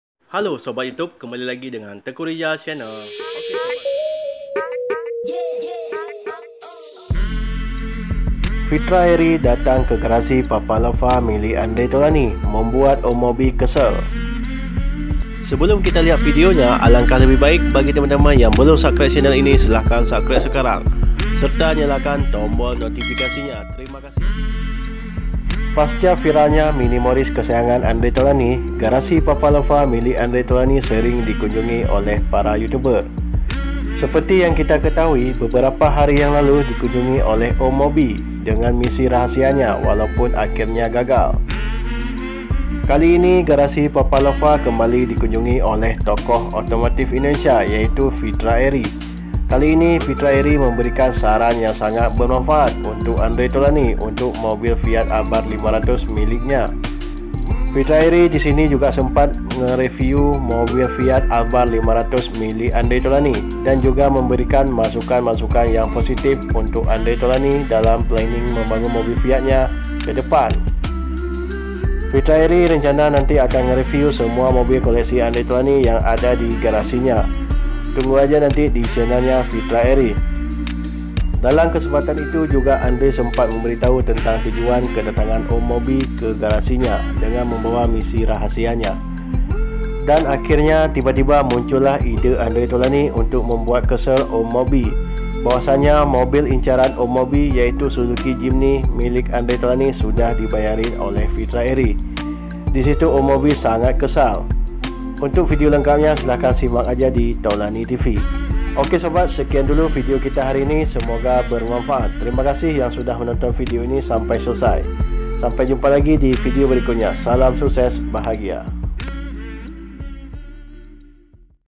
FITRA ERI DATANG KE GARASI PAPA LOVA MILIK ANDRE TAULANY MEMBUAT OM MOBI KESAL
Garasi viral papalova kali ini kedatangan tokoh otomotif fitra eri apa yang dilakukan fitra eri digarasi andre taulany simak video ini sampai selesai

Terima Kasih